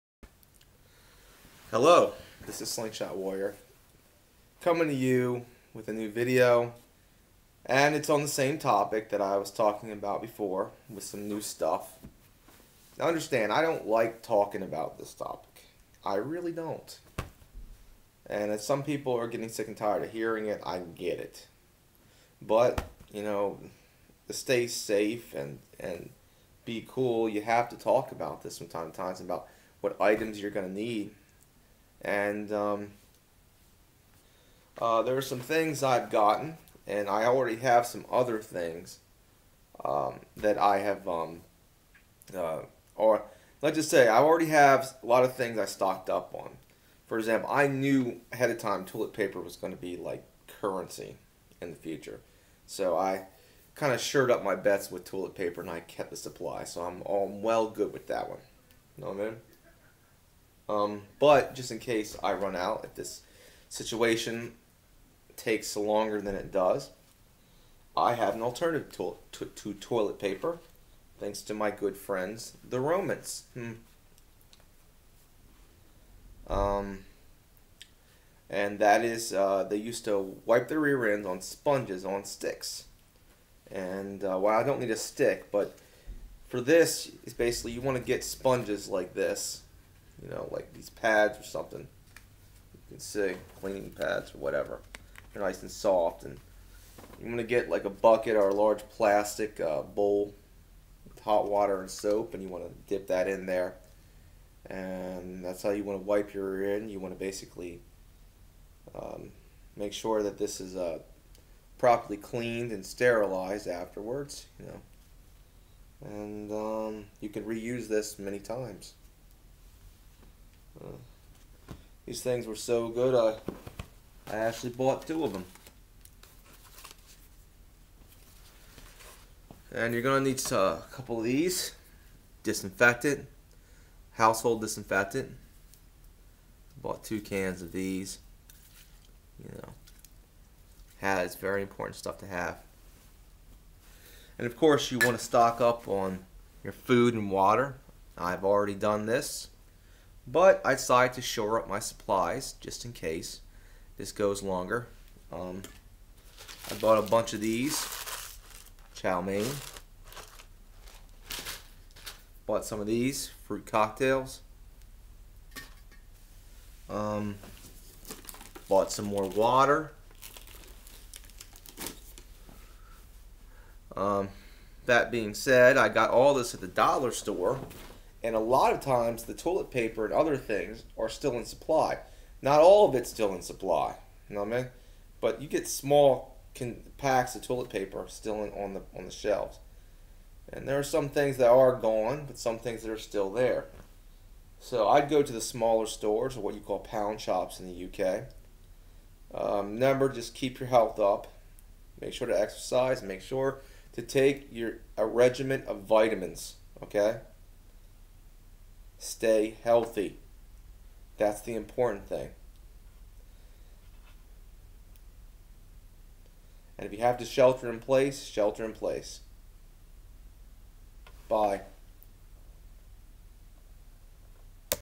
New Supplies for the pandemic!
It's me doing a new corona prepping video, these videos aren't fun to make but it has to be done.